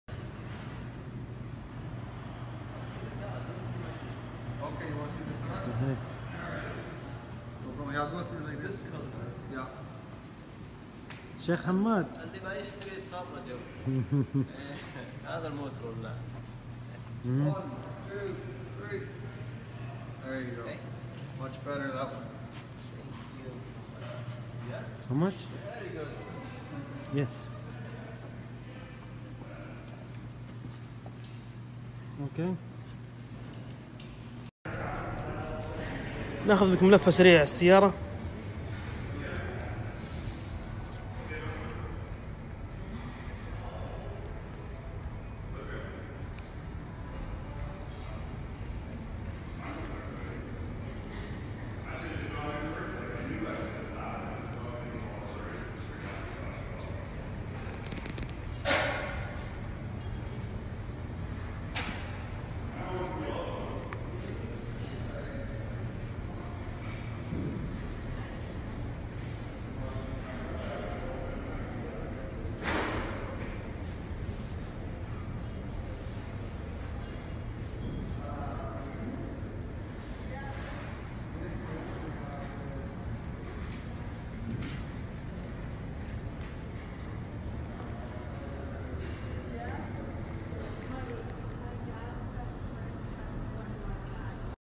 فخامة نادرة مرسيدس SL 300 .... تفعيل الاشتراك و🔔 لعروضنا القادمة
تنبيه 🚨 قرار التعامل مع اي شخص بيد المشاهد القناة لا تتحمل اي مسؤلية ...شكرا للجميع 🌹 تلقرام القناة :
الاعلان من الانستقرام للحساب: vip_classics